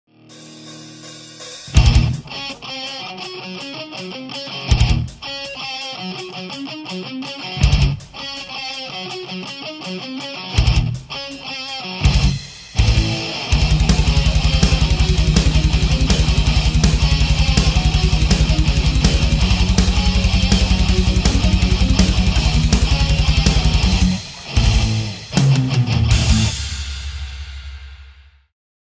SenSiMiLLia live - Online music festival 2020
Allow us to introduce our next band act  performing at online music festival 2020 @sensimillia.official . SenSiMiLLia is a 5 piece band founded in early 2012. SenSiMiLLia blends the melodies and agression of classic metal and modern metal sounds!!